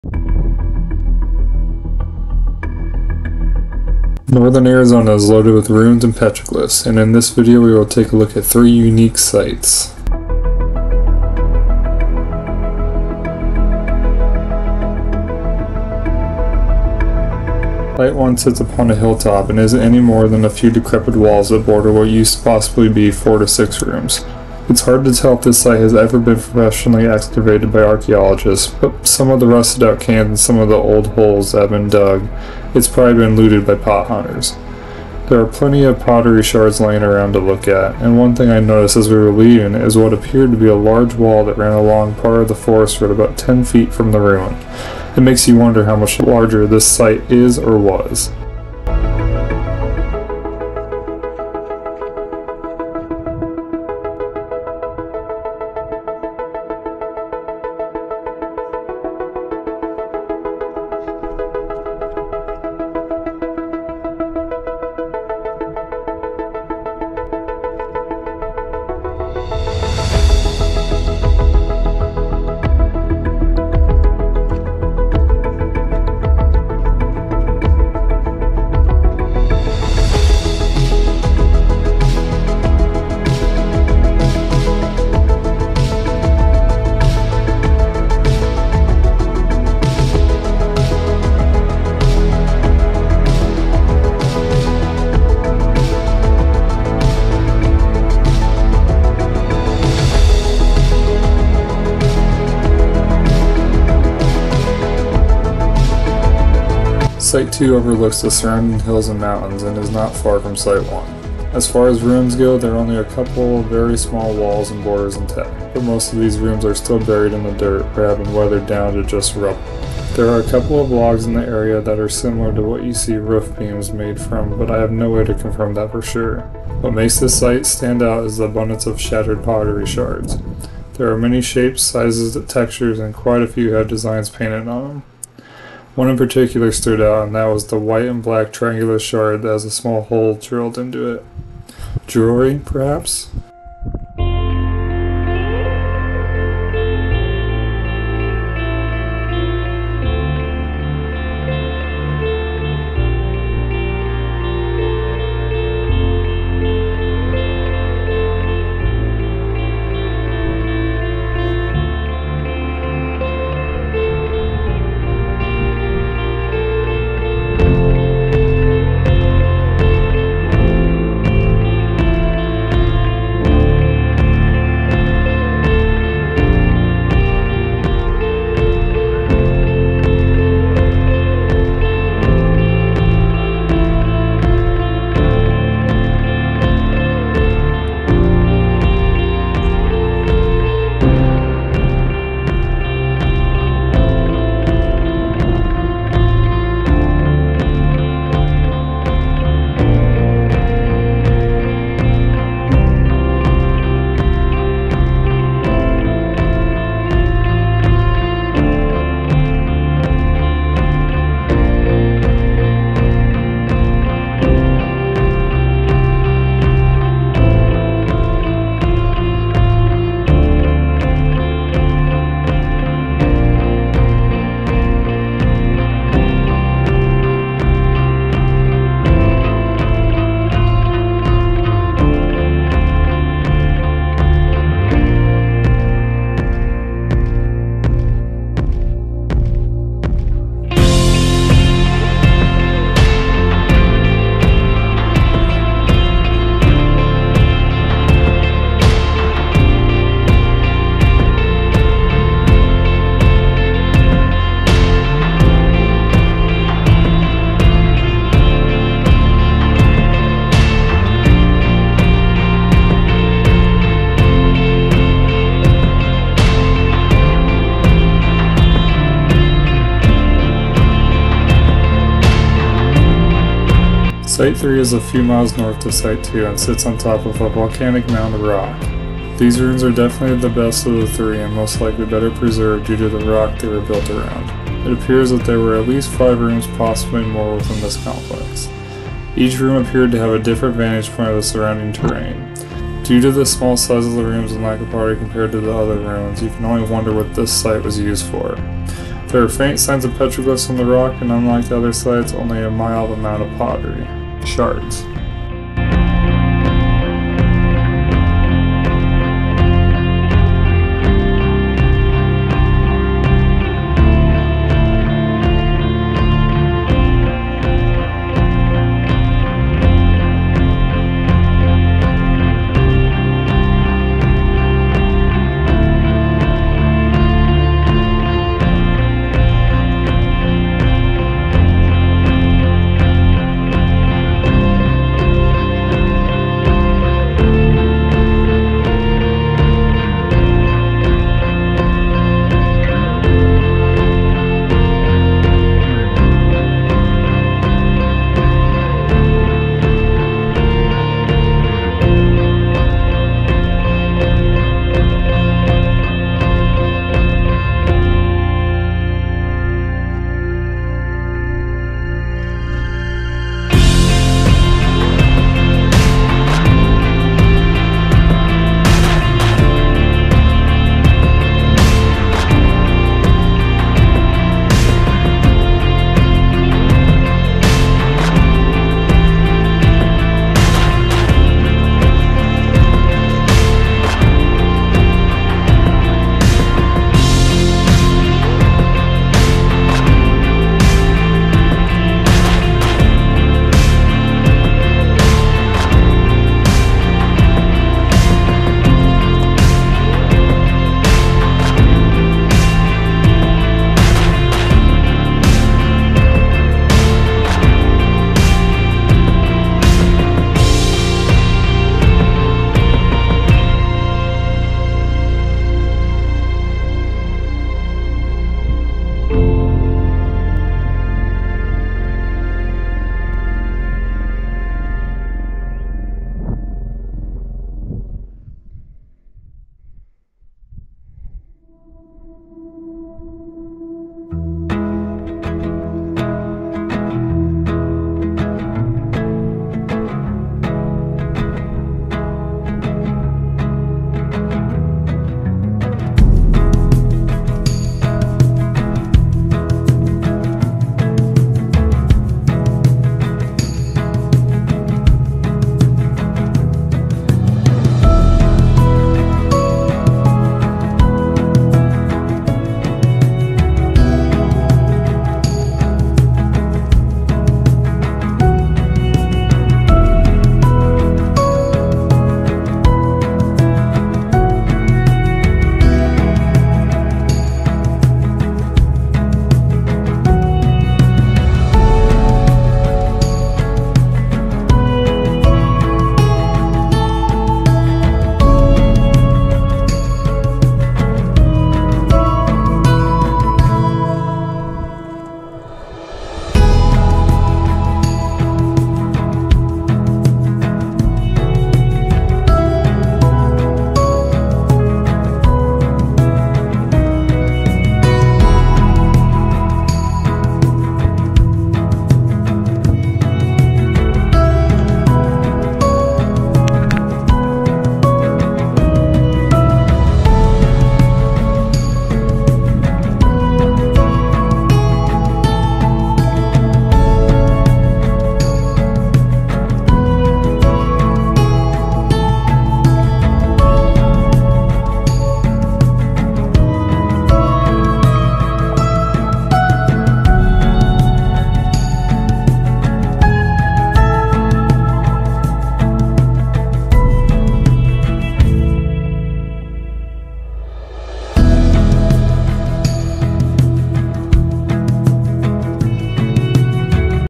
Exploring Ancient America, Ruins of Arizona - Three Unique Sites - Lonely Saguaro
In this video we explore three unique sites in northern Arizona.

The first site sits on top of a grassy hill top with only rubble left for walls, which creates an outline of what part of this ruin may have looked a 1000 years ago! There was a decent amount of broken pottery in the area and on the way out I noticed a large wall that was about 10-15ft from the ruin and was partially on the forest road. It makes you wonder how much bigger this site really is.

Site two, much like site one, only had a few piles of rock that created the borders of these rooms from the past. There were however two walls that formed a corner that appeared to be decently in tact. The biggest difference between this site and the first was the amount of pottery shards that lay all over the site and even a quarter plus mile away. There were a variety of shapes, textures, painted designs, and even a piece with what appeared to be a small hole drilled into it.

Site Three was the most intact ruin of the three. This site had four clearly defined rooms and another that was pretty worn down. Being hidden in the volcanic rock may have helped preserve some of these rooms unlike the other two sites that were completely exposed to the elements. This site also had a few petroglyphs nearby and a few pottery shards in the area.